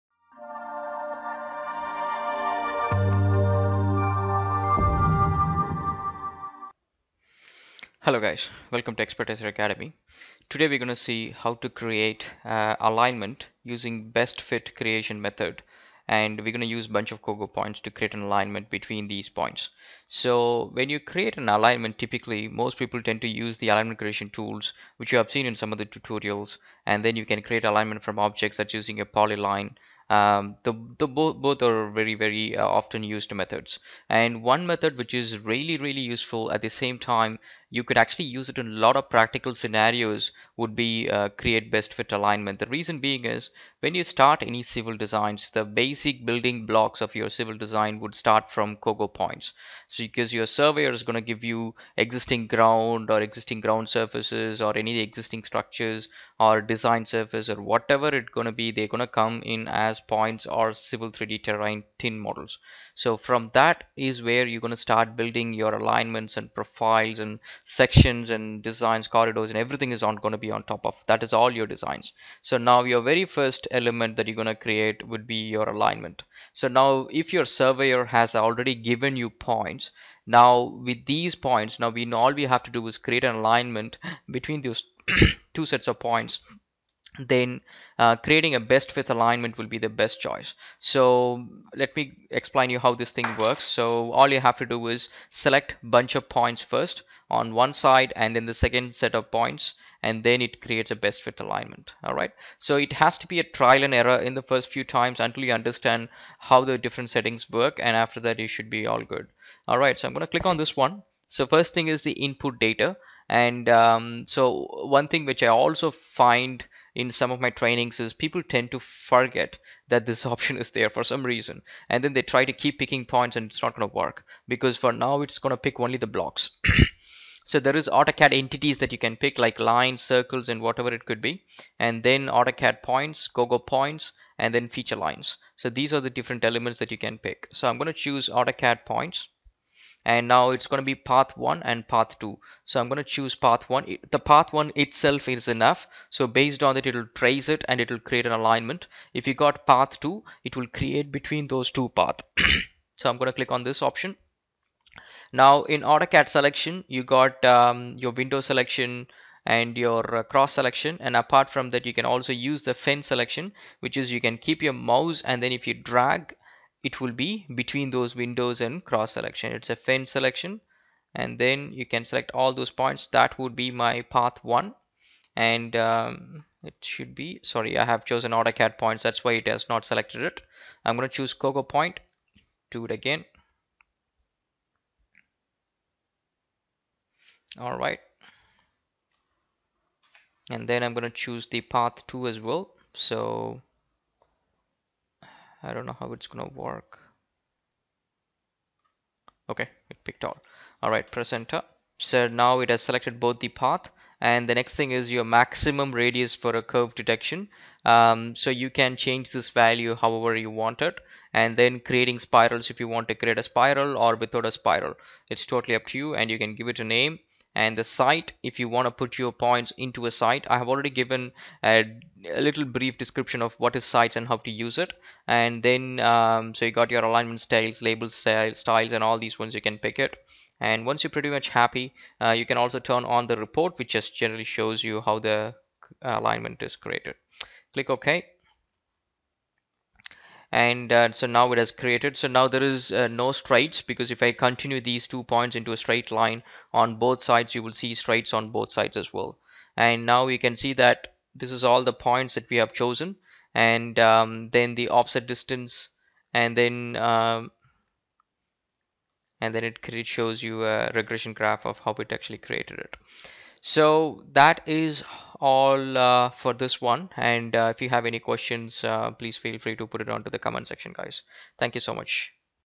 Civil3D Alignment Creation using Best Fit Method
Autodesk Civil3D 2018/ 2019
Autodesk, civil3d, tutorial

Civil3D Fundamentals Tutorial - Getting Started